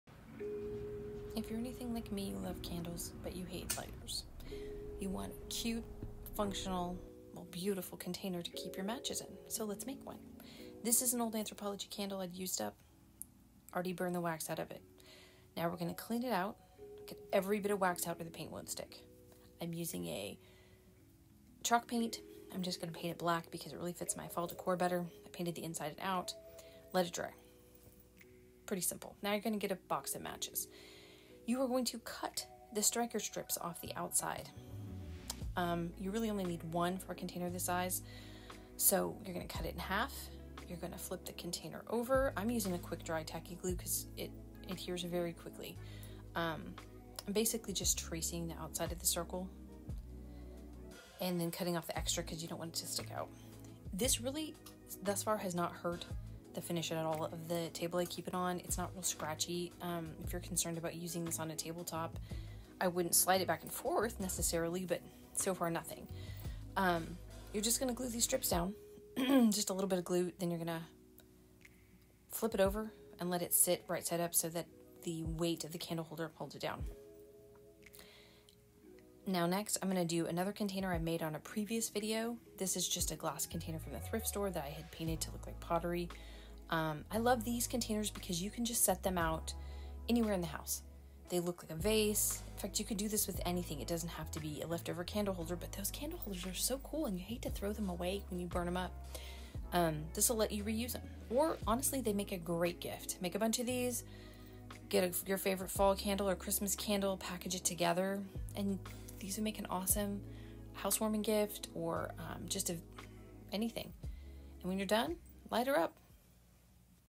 Anthropologie/Pottery Barn Candle Hack
I’ll show you how to make a fabulous match holder/lighter from an empty candle holder, used up candle, or just a neat decorative container.

You’ll need:
One smallish container
Large wooden matchbox
Tacky glue

If you have a used up candle clean it well inside and out. Paint won’t stick to wax! I chose to paint my container with black chalk paint to better match my fall and winter decor. Creativity is your friend here! Scour your home for small containers you could use - you might be surprised!

After your container is cleaned and dry ( if you painted it) flip it over and cut out the strike strips from your large box of matches. These are the sand paper like strips on the side of the box. Use your tacky glue or super glue to adhere these to the bottom of your container….flip it over and fill it with wooden matches. Place near your favorite holiday candles and enjoy your creativity!

GIFT IDEA: Make several match holders and buy corresponding holiday candles. Package your match holder and candle together for a fun holiday gift, housewarming present or hostess gift!